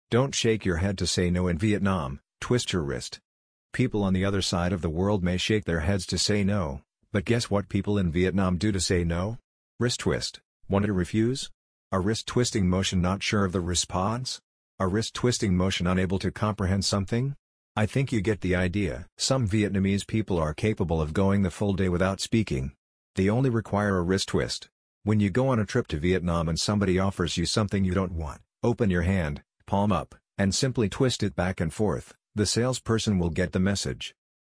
Don't shake your head to say no in Vietnam, twist your wrist
People on the other side of the world may shake their heads to say no, but guess what people in Vietnam do to say no? Wrist twist.